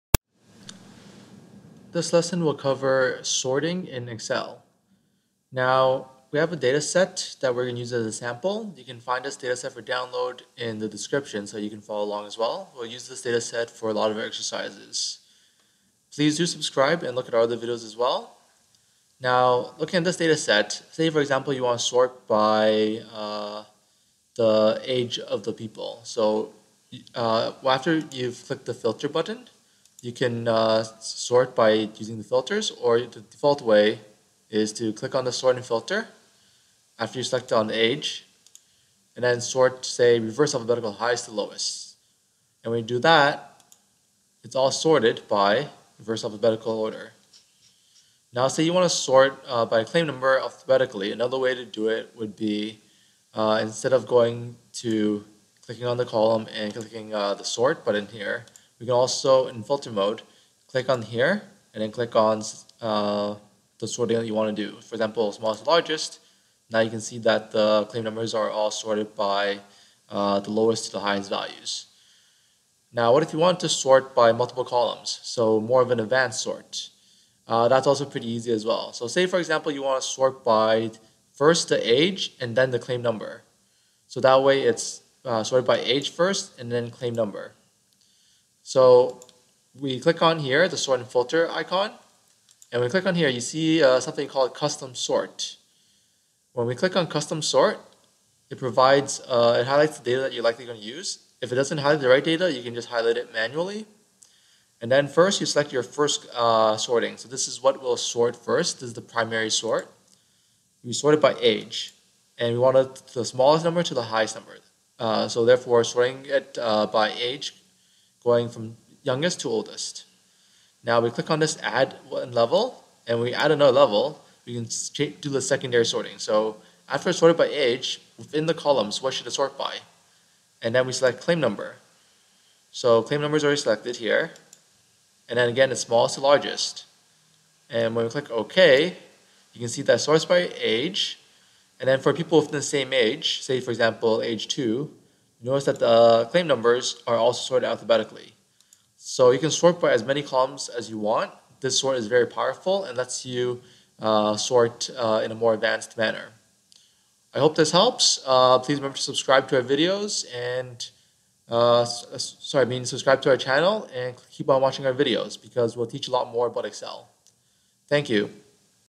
Actuarial Excel - How to Sort in Excel, sort multiple columns, advanced sort
In this lesson we cover how to sort in Excel. Actuarial science perspective.

You'll learn:
-sorting
-filter sorting
-advanced sorting (sort multiple columns)